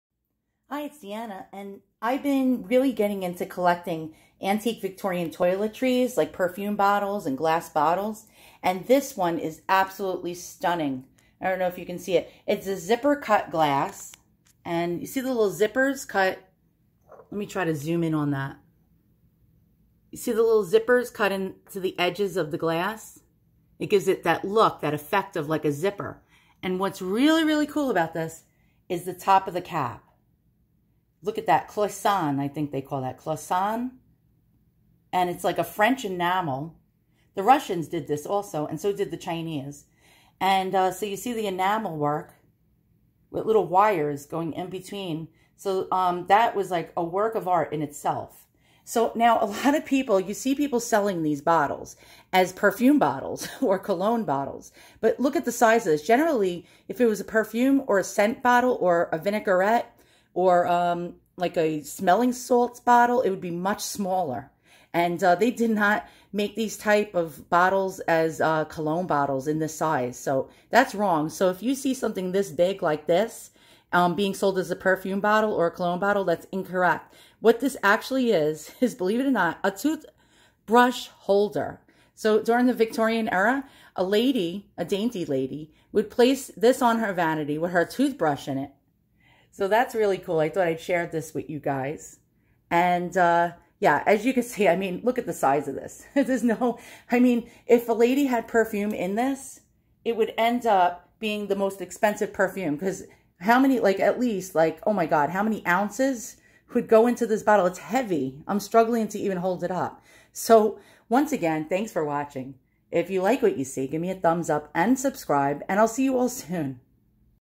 Antique Art Nouveau Zipper Cut Glass Champleve Bottle~Talc Powder Or Toothbrush Holder Vanity Bottle
***UPDATE***: The top enamel work on the cap is called Champleve. I always get Champleve mixed up with Cloisonne. Sorry for the error.
This beautiful old gut glass crystal bottle was sold as a ladies vanity item. It was used to hold tooth brushes or talc powder. A lot of people selling these think these are perfume or cologne bottles.